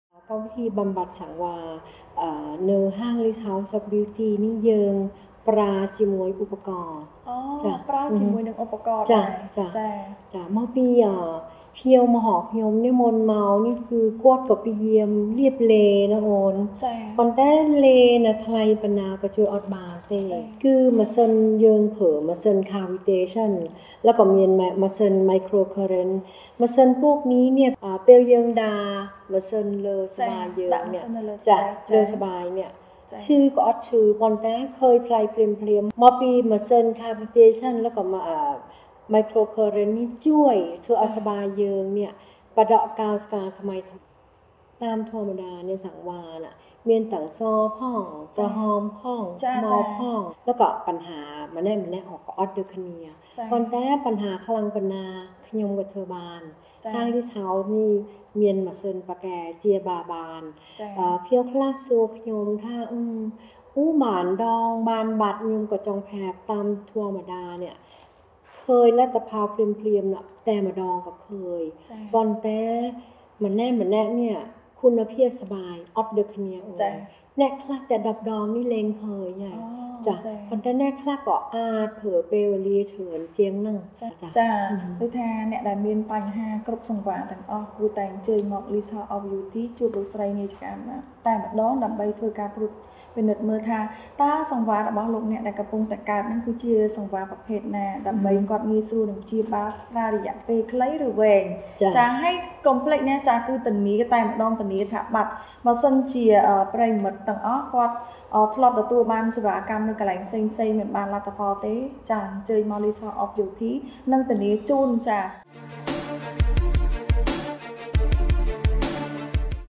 😪😪 ឈប់ព្រួយបារម្ភអំពីបញ្ហាសង្វារ ទោប្បីបែកខ្លាំងនិងក្រាសប៉ុណ្ណាក៏ដោយ
👉👉 លីហ្សហៅស៏អហ្វប៊ីយូធី ជំនាញឯកទេសព្យាបាលលុបបំបាត់ស្លាកស្នាមសង្វារជាមួយម៉ាស៊ីន Ultrasound។
💝💝 ត្រឹមតែម្តងនឹងទទួលបានលទ្ធផលភ្លាមៗ
💝💝 មិនមានការវះកាត់ មិនមានការឈឺចាប់
💝💝 មិនប្រើប្រាស់ផលិតផលគីមី មិនមានផលប៉ះពាល់ដល់សុខភាព